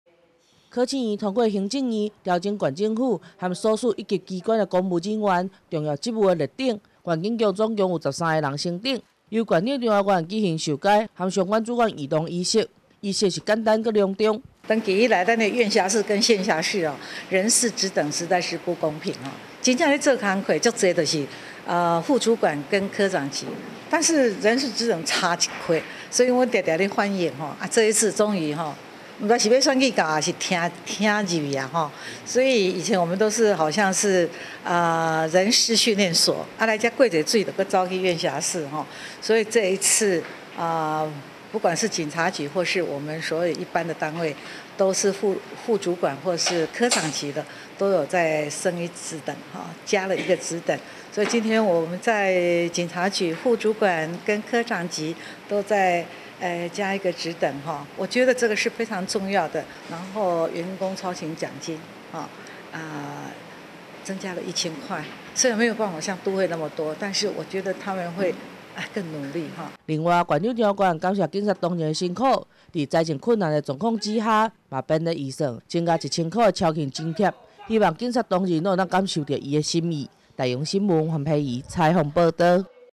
20180725大揚新聞 留住人才 縣警局科長等職務等階提升授階
考試院通過行政院調整縣政府及所屬一級機關公務人員重要職務列等及相對應的警察官職務等階，縣警局有保安科長林慶珍等13人等階提升，由縣長張花冠舉行授階及相關主管異動、陞職儀式。